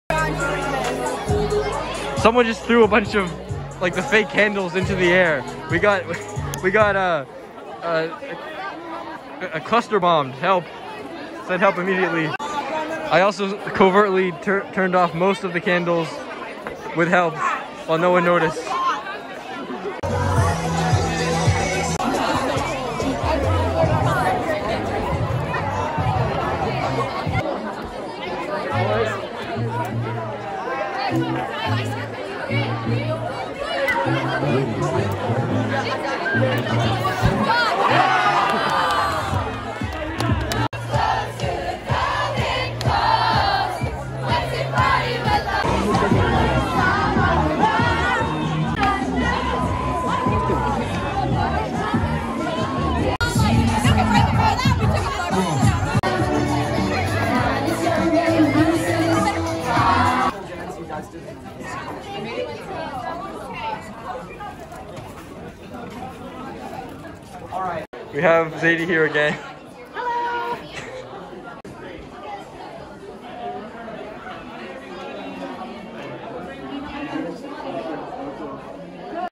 Jeremy Fitzgerald Guards Dance at Freddy Fazbear’s Pizza
Spider-Man: Homecoming Dance
Filmed on Friday the 14th.

I used to blow out the candles on tables at restaurants when I was little, then graduated to turning off fake ones.  I have carried on that tradition here.
I couldn’t find any pen or knife, so I used a plastic spoon with a broken and thus, sharp, handle that my friend found to violently jab eye holes in the box.